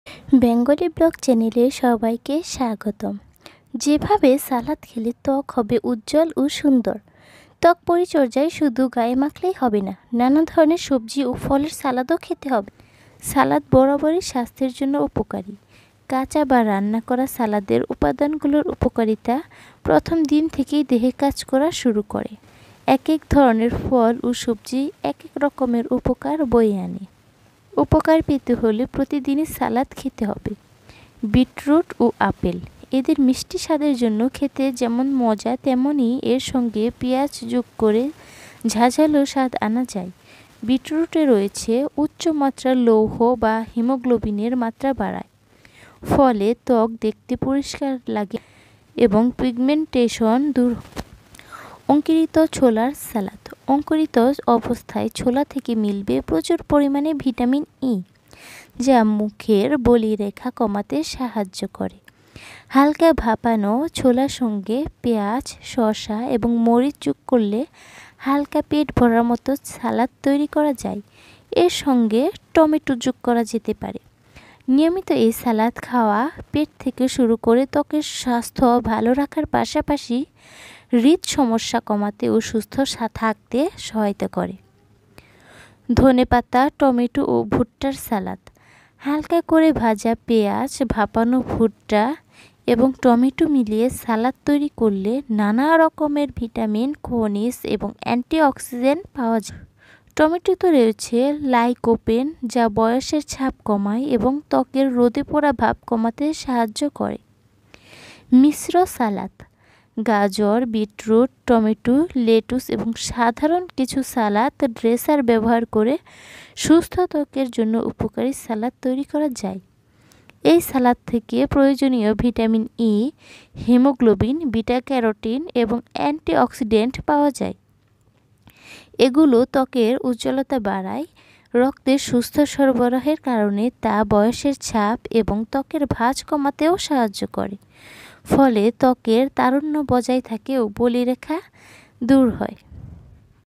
ত্বক ফর্সা করার সালাদ।ত্বকের ফেতর থেকে ফর্সা ও উজ্জ্বল করবে।মুখের দাগ,ব্রণ দূর করবে।*Bangali blog.
সালাদ খেয়ে ত্বক করুন ফর্সা।ত্বক ফর্সা করার সালাদ।ফর্সা হওয়ার জন্য শুধু গায়ে মাখলেই হবে না,খেতেও হবে।আজকে আপনাদের সাথে সেই খাবার গুলো নিয়ে আলোচনা করা হলো।